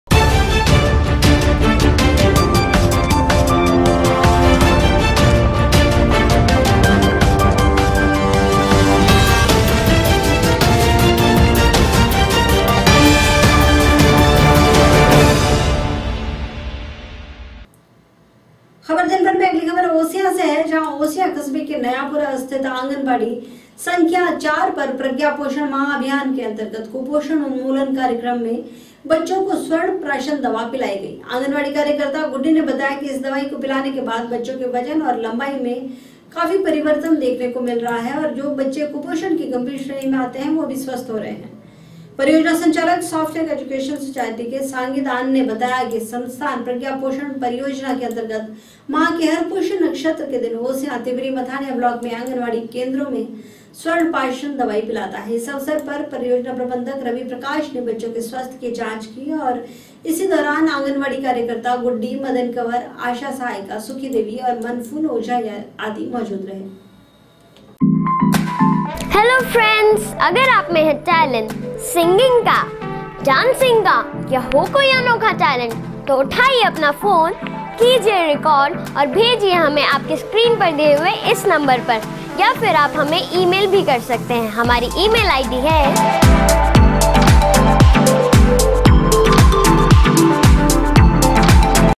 ख़बर दिनभर ओसियां
ओसियां कस्बे के नयापुरा स्थित आंगनबाड़ी केंद्र संख्या 4 पर प्रज्ञा पोषण महाअभियान के अंतर्गत कुपोषण उन्मूलन कार्यक्रम के तहत बच्चों को स्वर्ण प्राशन दवा पिलाई गई। आंगनवाड़ी कार्यकर्ता गुड्डी ने बताया कि इस दवाई को पिलाने के बाद बच्चों के वजन तथा लंबाई में बहुत परिवर्तन देखने को मिला और जो बच्चे कुपोषण की गंभीर श्रेणी में आते है वो भी स्वस्थ हो गए हैं।परियोजना  संचालक सॉफ्टटेक एजुकेशन सोसायटी के सचिव सांगीदान पालीवाल ने बताया कि संस्थान प्रज्ञा पोषण परियोजना के अंतर्गत माह के हर पुष्य नक्षत्र के दिन ओसियां,तिंवरी, मथानिया ब्लॉक में आंगनवाड़ी केंद्रों पर स्वर्ण प्राशन दवाई पिलाई जाती है, इस अवसर पर  परियोजना प्रबंधक डॉ रवि प्रकाश ने बच्चों के स्वास्थ्य कि जांच की। इसी दौरान आंगनवाड़ी कार्यकर्ता गुड्डी, मदन कंवर, आशा सहायिका सुखी देवी,मनफूल ओझा मौजूद रहे।